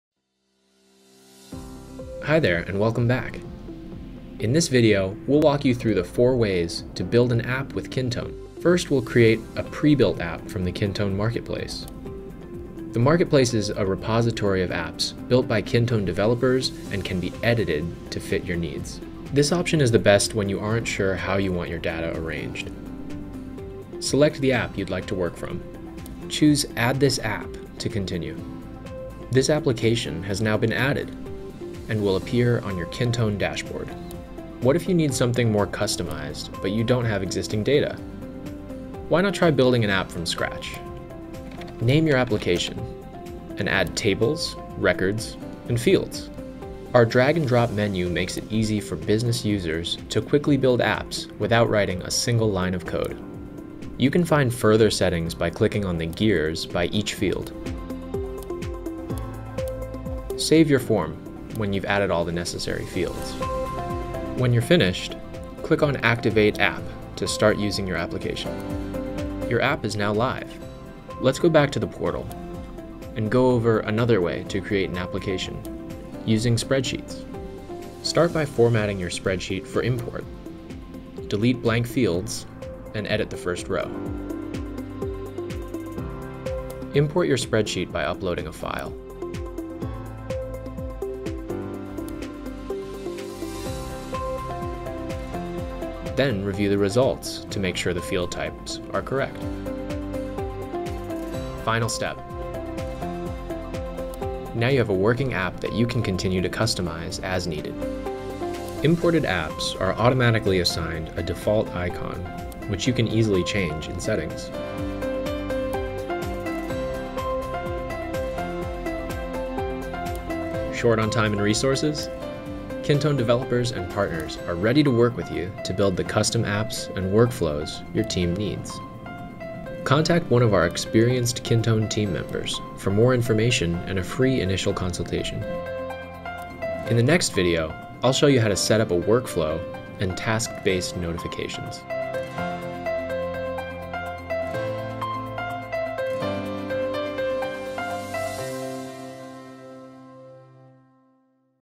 3. Building an App with Kintone
To code or not to code—it’s totally up to you.

Become the office hero. Build bespoke enterprise apps for your team and zap them to every desk and pocket. Automate just about anything: start from scratch with drag-and-drop, or customize one of dozens of pre-built apps in millions of ways. Collaborate with your team to run, test, and improve until your app is picture-perfect and super impressive—all without a lick of code. (We won’t tell if you don’t).